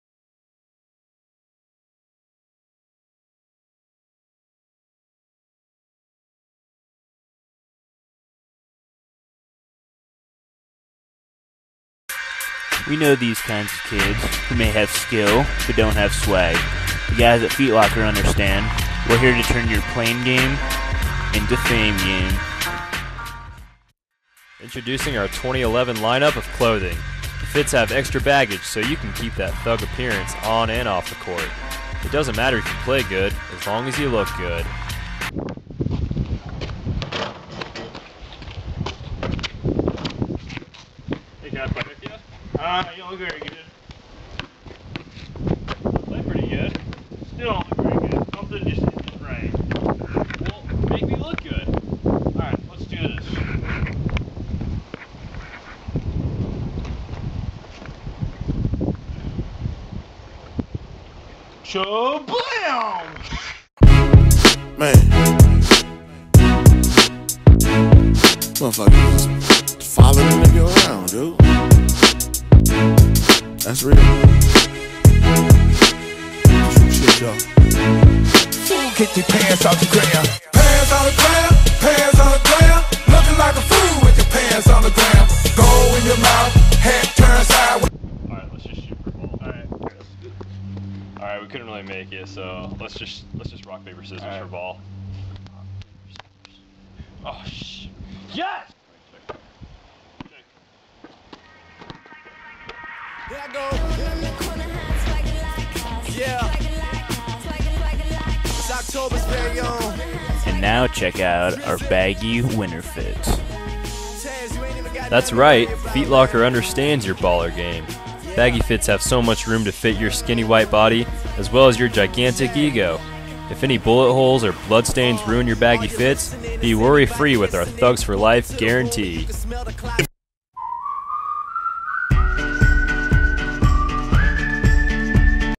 Baggy Fit$$.mp4
Mlive and Aheit in a parody advertisement